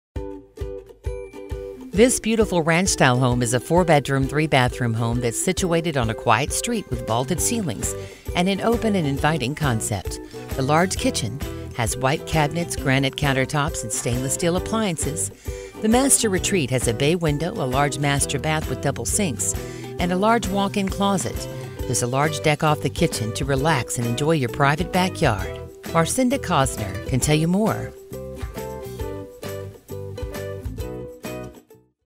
Marcyndah Cosner presents 3354 South Nelson Street Lakewood, CO | ColdwellBankerHomes.com
3354 South Nelson Street, Lakewood, CO

This beautiful ranch style home in Pheasant Creek at The Bear is waiting for your personal touches. It is a four bedroom, three bathroom home that is situated on a quiet street. As you enter, you are greeted with vaulted ceilings and an open and inviting concept. The large eat-in kitchen has white c